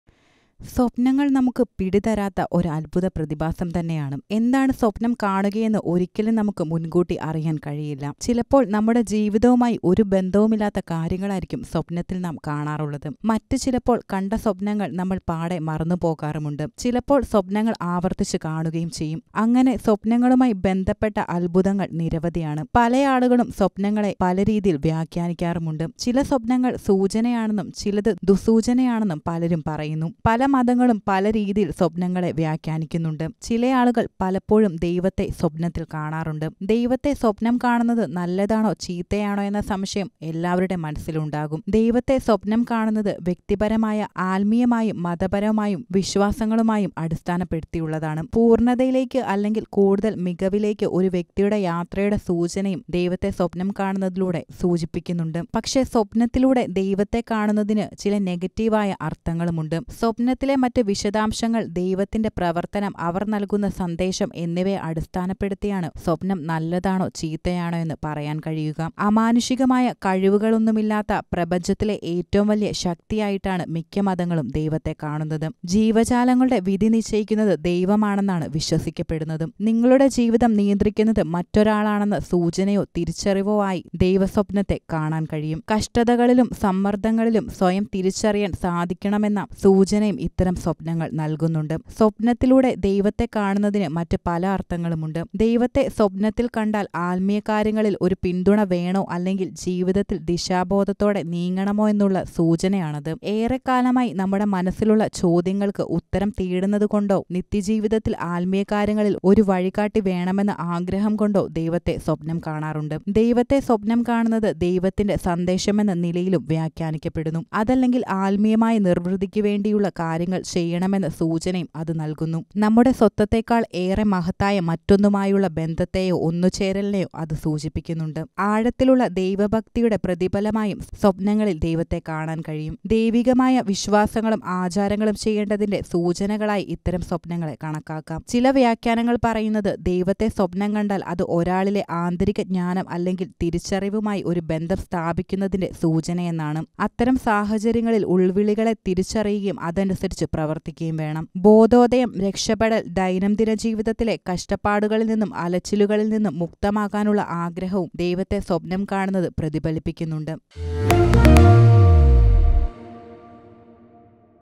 ദൈവം സ്വപ്നത്തിലൂടെ നിങ്ങളോട് പറയുവാൻ ആഗ്രഹിക്കുന്നത്.. | God Dreaming
ചിലപ്പോള്‍ ജീവിക്കുന്ന ജീവിതവുമായി ഒരു ബന്ധവുമില്ലാത്ത കാര്യങ്ങളായിരിക്കും നമ്മൾ സ്വപ്‌നത്തില്‍ കാണുക. മറ്റുചിലപ്പോള്‍ കണ്ട സ്വപ്‌നങ്ങള്‍ എല്ലാം പാടേ മറന്നുപോകും. ചിലപ്പോള്‍ സ്വപ്‌നങ്ങള്‍ ആവര്‍ത്തിച്ച് കാണും. അങ്ങനെ സ്വപ്‌നങ്ങളുമായി ബന്ധപ്പെട്ട അത്ഭുതങ്ങള്‍ വളരെ ഏറെയാണ്. സ്വപ്‌നങ്ങളെ വ്യാഖ്യാനിക്കുന്നവരുണ്ട്. ചില സ്വപ്‌നങ്ങള്‍ ശുഭസൂചനയാണെന്നും ചിലത് ദുസ്സൂചനയാണെന്നും കണക്കാക്കാറുണ്ട്. പല മതങ്ങളും പല രീതിയില്‍ സ്വപ്‌നം കാണുന്നതിനെ വ്യാഖ്യാനിക്കാറുണ്ട്...